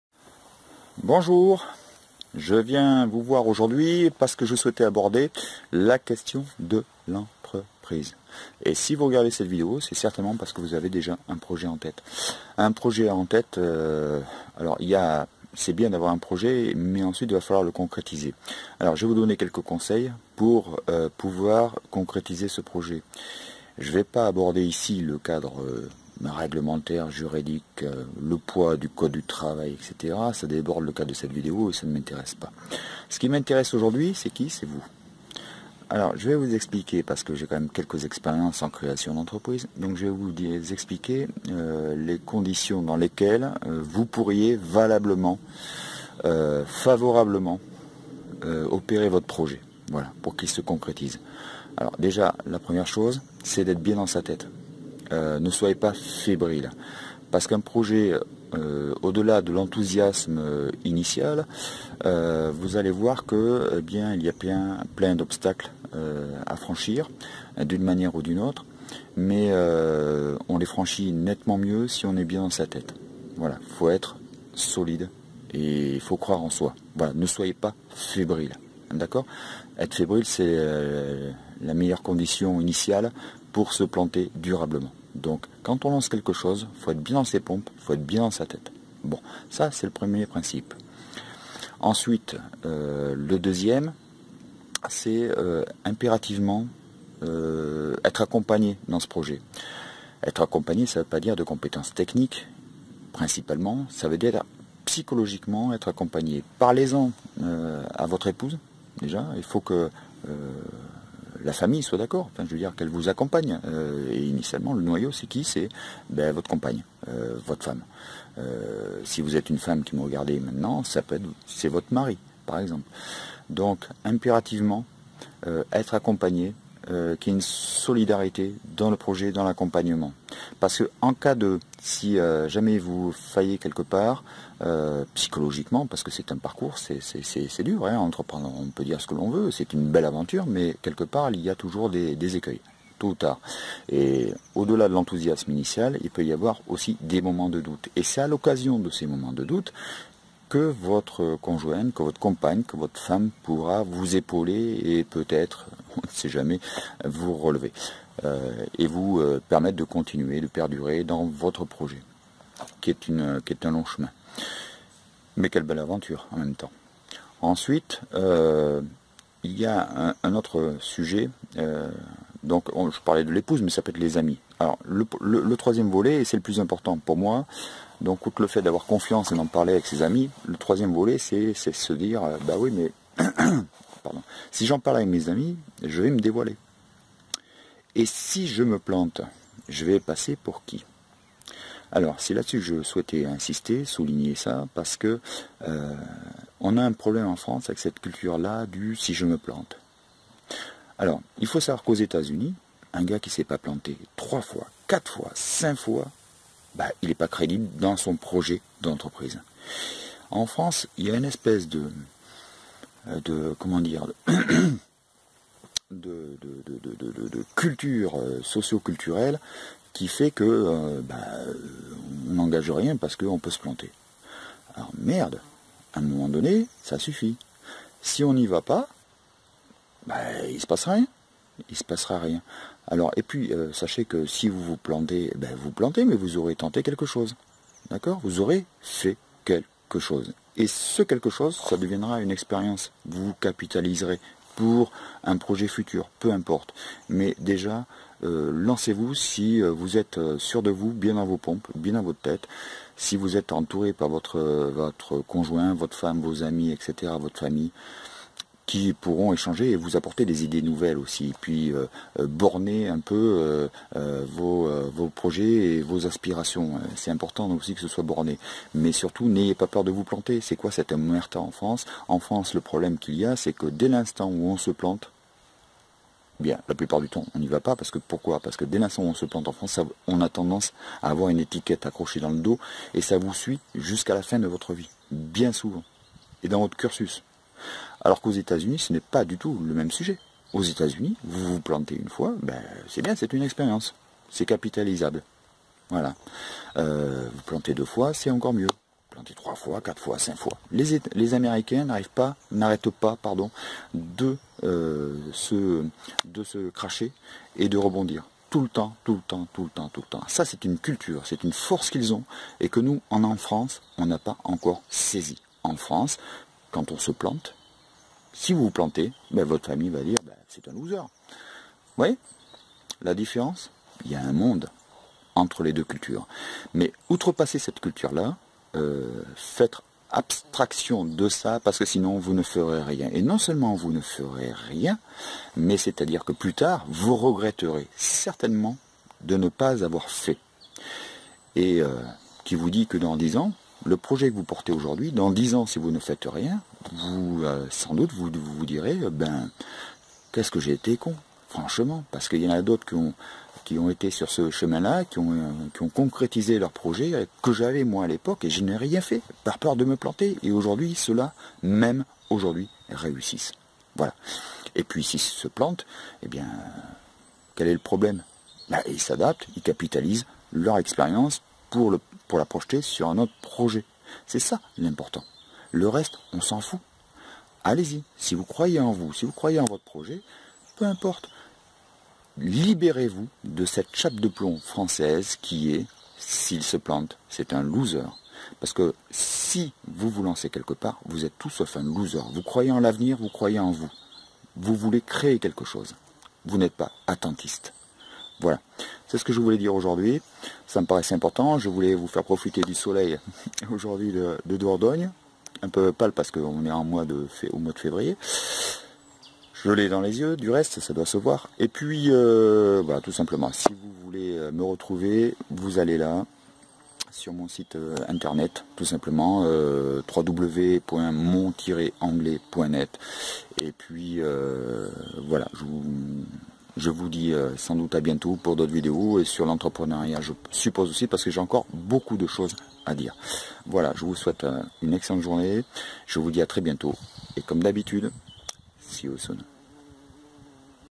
Créer son entreprise | les 3 clés !
Dans cette vidéo, je vous donne 3 principes clés à respecter avant de vous lancer dans l'aventure de l'entreprise.